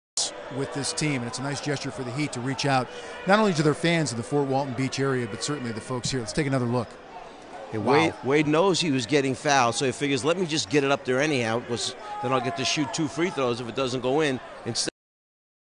Dwyane Wade - Circus Shot (Heat Real Training Camp)
Dwyane Wade hits the circus shot as the Heat scrimmage.